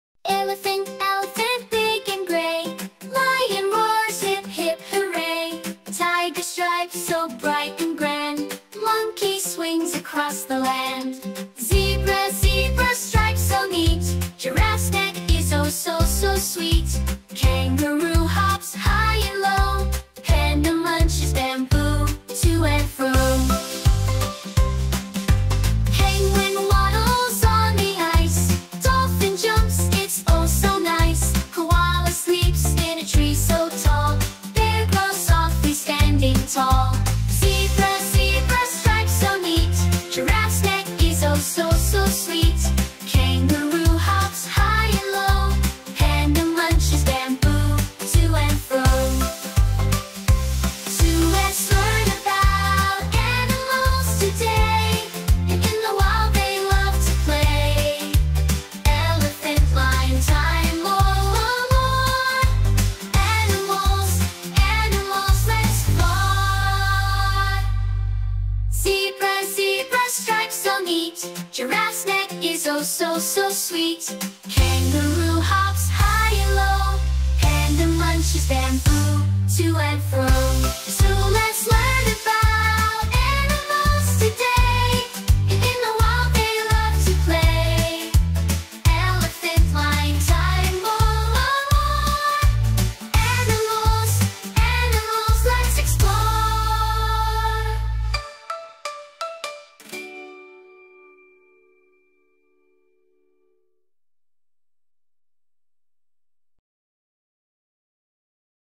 Fun Animal Songs for Kids | Fun Nursery Rhymes for Kids
"Get ready for a musical adventure with our Fun Animal Song! This catchy tune invites kids to sing along and learn about their favorite animals. With lively melodies and engaging visuals, your little ones will enjoy discovering different animals and their sounds. Perfect for sing-alongs at home or in the classroom!"